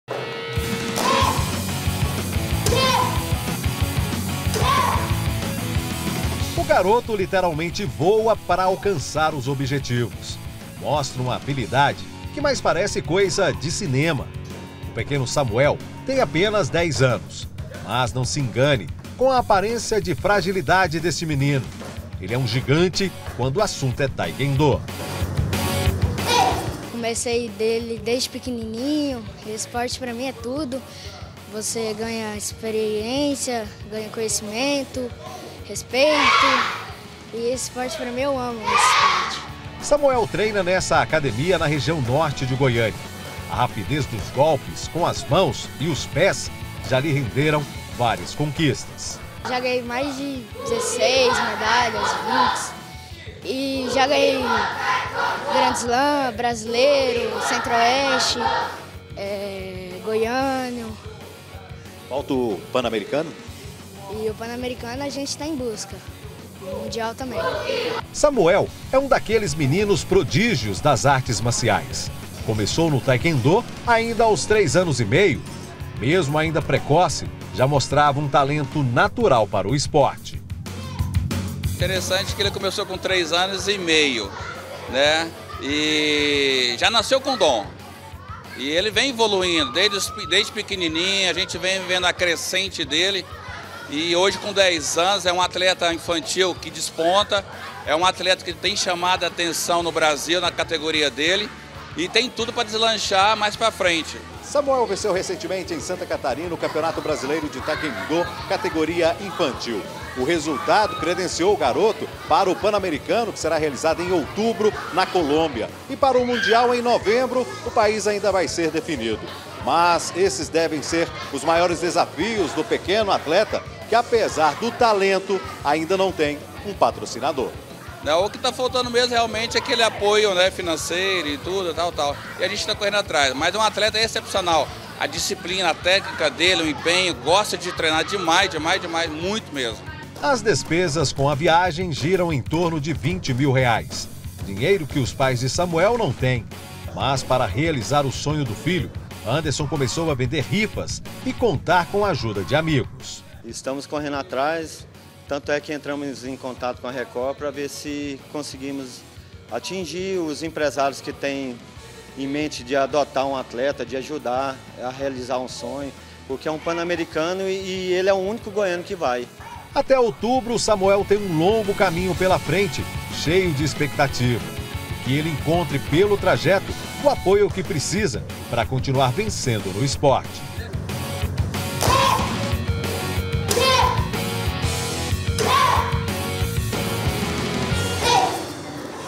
MENINO TAEKWONDO - GOIANO DE 10 ANOS VAI DISPUTAR O PANAMERICANO
TAEKWONDO - GOIANO DE 10 ANOS VAI DISPUTAR O PANAMERICANO
UM GOIANINHO DE 10 ANINHOS ACABA DE VENCER O CAMPEONATO BRASILEIRO, E VAI DISPUTAR O PANAMERICANO DE TAEKWONDO.// MAS A FAMÍLIA NÃO CONSEGUE PAGAR A VIAGEM PARA A COLÔMBIA, E LUTA CONTRA O TEMPO PARA CONSEGUIR 20 MIL REAIS ATÉ NOVEMBRO.//